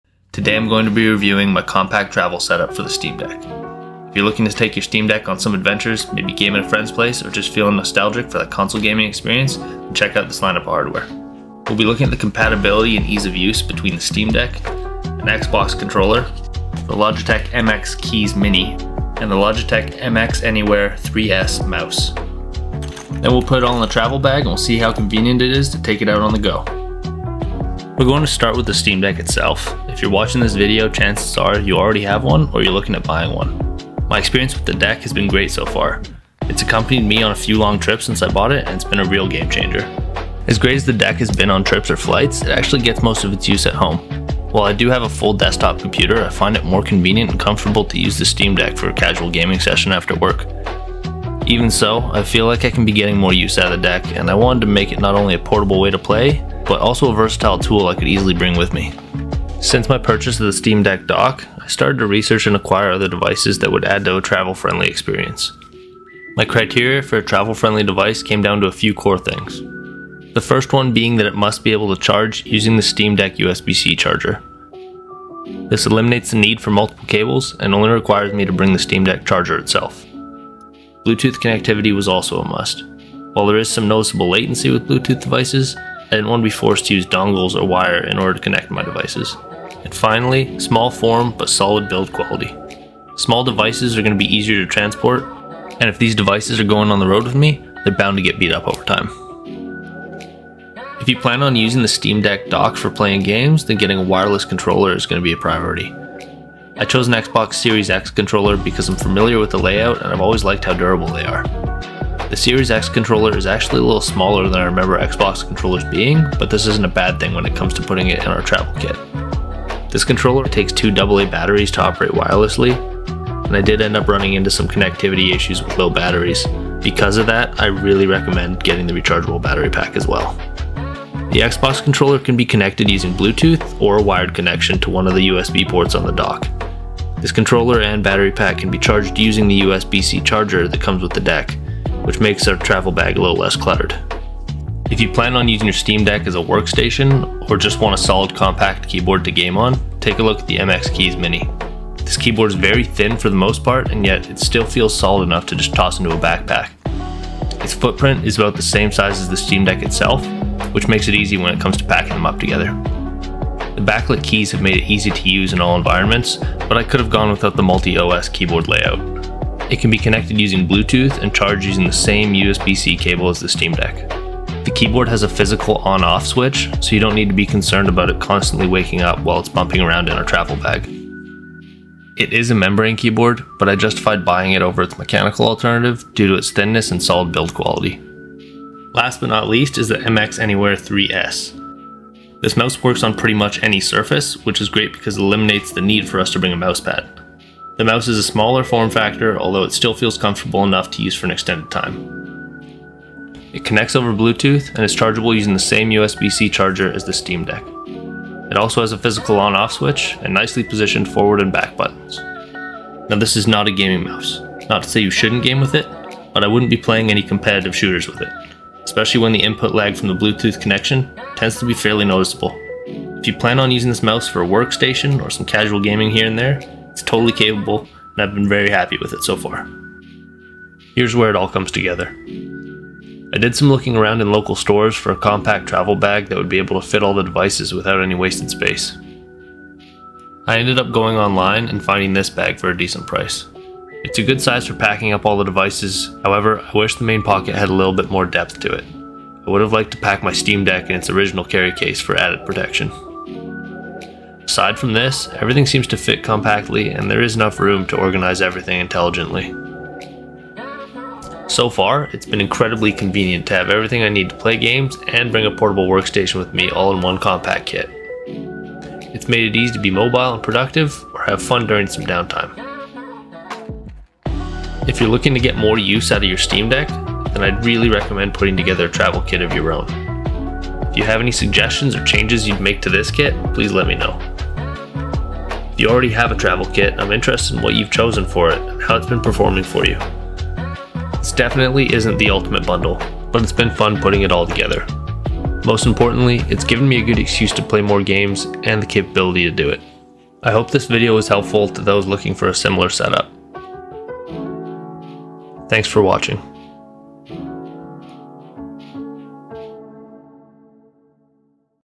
Steam Deck Travel Kit
In this video I bring together a collection of peripheral devices to create a travel kit for my Steam Deck and show off some of the reasons behind their selection. If you're looking at expanding the use of your Steam Deck but want to keep it portable and convenient then you may be interested in checking out these accessories.